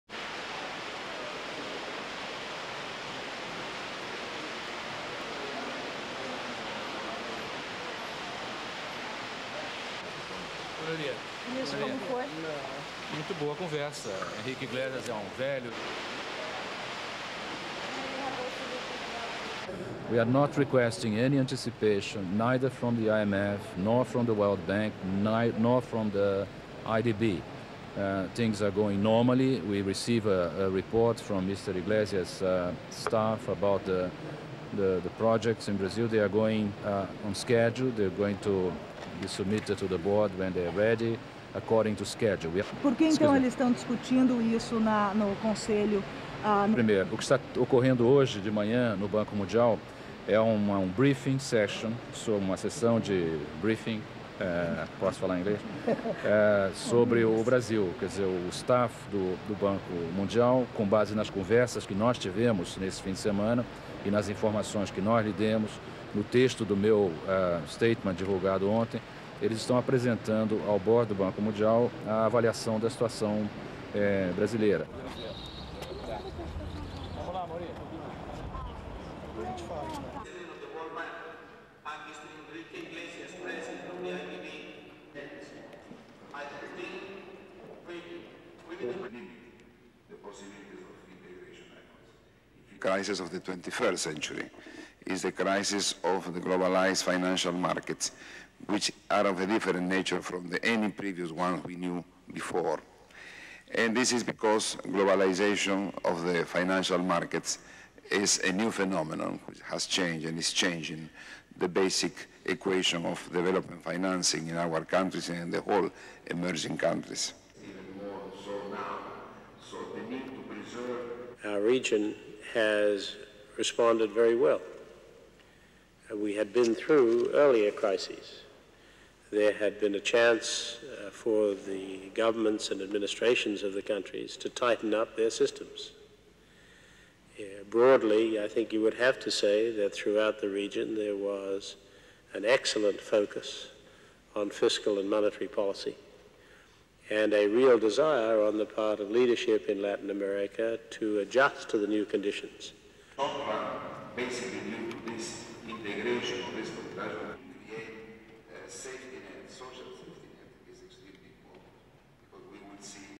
USA: BRAZILIAN FINANCE MINISTER MALAN MEETS IDB LEADERS
(19 Jan 1999) Eng/Portu/Nat

Brazil's Finance Minister, Pedro Malan, was optimistic as he wrapped up three days of talks with international financial institutions in Washington.

His last meeting was with leaders of the Inter- American Development Bank (I-D-B) on Tuesday,

He said Brazil had not asked for any international organization for financial help in dealing with its currency crisis.

Brazilian stocks were higher on Tuesday after a turbulent week.

Malan's meeting with the I-D-B came after three days of meetings with the International Monetary Fund (I-M-F), the U-S Federal Reserve and the World Bank.

Malan insisted that Brazil's fiscal policies were on schedule.

He said the country needed no additional help to regain economic footing after allowing its currency to float on world markets last week.

SOUNDBITE: (English)
"We are not requesting any anticipation neither from the IMF, nor from the World Bank, nor the I-D-B. Things are going normally, we receive a report from Mr. Iglesias' staff about the projects in Brazil, they are going on schedule. They're going to be submitted to the board when they are ready, We are not requesting any anticipation."
SUPER CAPTION: Pedro Malan, Brazilian Finance Minister

Malan said the purpose of talks with the World Bank was to explain how his government will make its promised fiscal reforms.

SOUNDBITE: (English)
"They're receiving a briefing from staff of the bank on basis of discussions that we had with World Bank staff over the weekend. They will be explaining text of my policy statement made public yesterday."

In a briefing on Tuesday hosted by the Organisation of American States (O-A-S) , leaders from Latin America and the Caribbean expressed concern about the latest economic crisis to hit the region.

The President of the Inter-American Development Bank, Enrique Iglesias, said that although the crisis originated in Asia, the effects of the global economy also had repercussions for Latin America.

SOUNDBITE: (English)
"The crisis of the 21st century is the crisis of the globalised financial markets, which are of a different nature than any of the other ones we knew before. And this is because the globalisation of world markets is a new phenomenon that has changed and is changing the basic equation of development financing in our countries and in the whole emerging countries."
SUPER CAPTION: Enrique Iglesias, President, Inter-American Development Bank

But the President of the World Bank, James Wolfensohn, was optimistic about Latin America's response to the financial crisis in the region.

SOUNDBITE: (English)
"Our region has responded very well. And we have been through earlier crises. There has been a chance for the governments and administrations of the countries to tighten up their systems. Broadly, I think you would have to say that throughout the region there was an excellent focus on fiscal and monetary policy. And a real desire on the part of the leadership of Latin America to adjust to the new conditions."
SUPER CAPTION: James Wolfensohn, President of the World Bank

Wolfensohn also praised Brazilian President Fernando Henrique Cardoso and Malan for their strong response to the economic crisis.

The crisis began last week with the new speculative attack on Brazil's currency, the real.